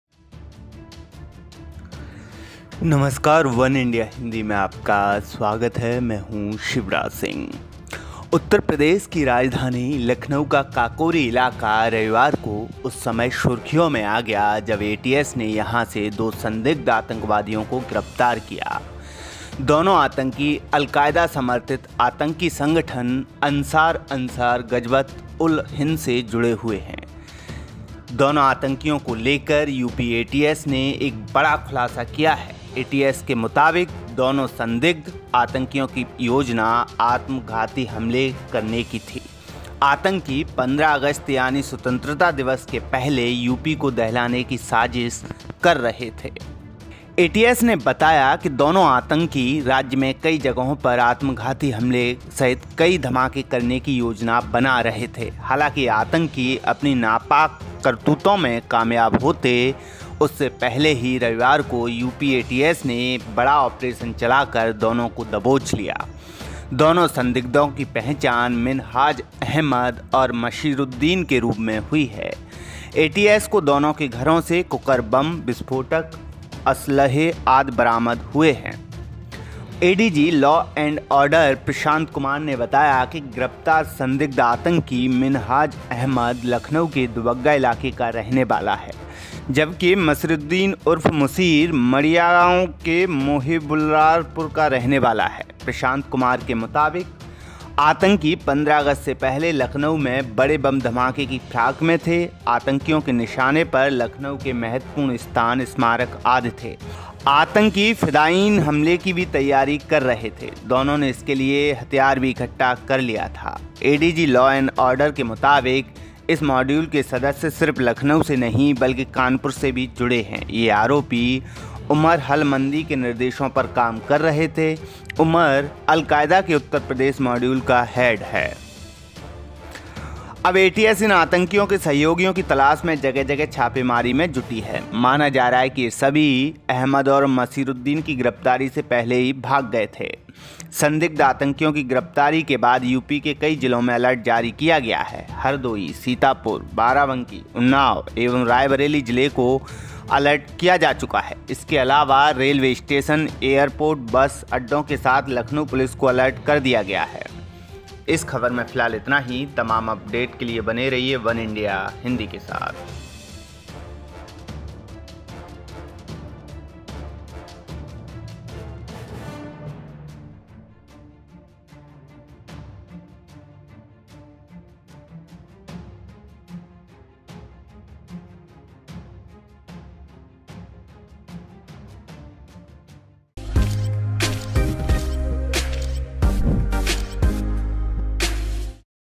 Lucknow Terrorist Arrested: UP को दहलाने की थी साजिश, ATS का चौंकाने वाला खुलासा | वनइंडिया हिंदी
The Kakori area of ​​Uttar Pradesh's capital Lucknow came into limelight on Sunday when the ATS arrested two suspected terrorists from here. UP ATS has made a big disclosure about both the arrested terrorists. According to the ATS, both the suspected terrorists had plans to carry out suicide attacks. Terrorists were conspiring to shake UP on 15 August i.e. before Independence Day.

उत्तर प्रदेश (Uttar Pradesh) की राजधानी लखनऊ का काकोरी इलाका रविवार को उस समय सुर्खियों में आ गया, जब एटीएस (UP ATS) ने यहां से दो संदिग्ध आतंकवादियों (Two Terrorist) को गिरफ्तार किया. गिरफ्तार दोनों आतंकियों को लेकर यूपी एटीएस ने बड़ा खुलासा किया है. एटीएस के मुताबिक, दोनों संदिग्ध आतंकियों की योजना आत्मघाती हमले करने की थी। आतंकी 15 अगस्त यानि स्वतंत्रता दिवस के पहले यूपी को दहलाने की साजिश कर रहे थे।